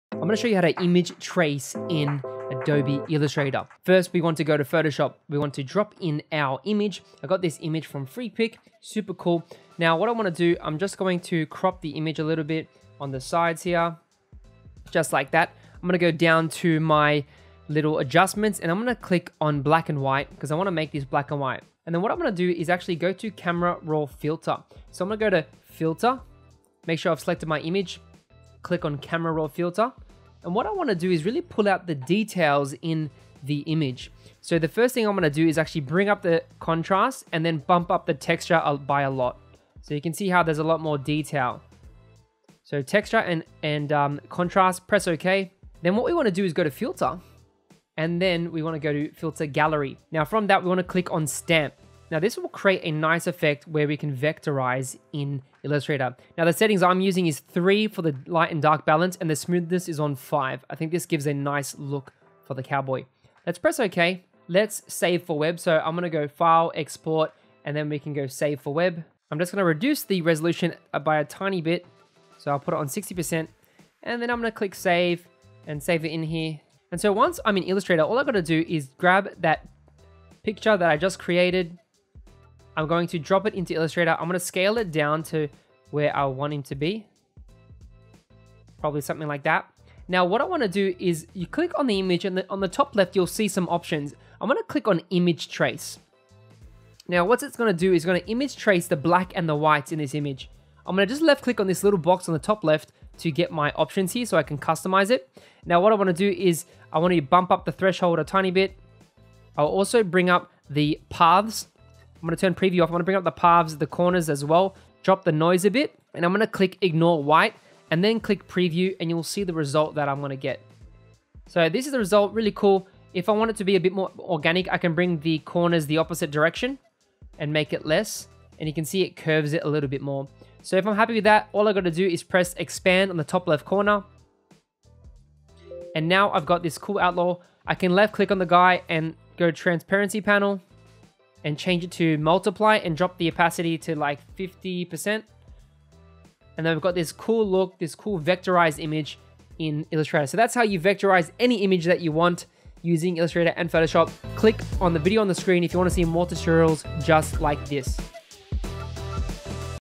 How to trace ANY image in adobe illustrator
I show you how to trace ANY image in adobe illustrator, in this quick tutorial.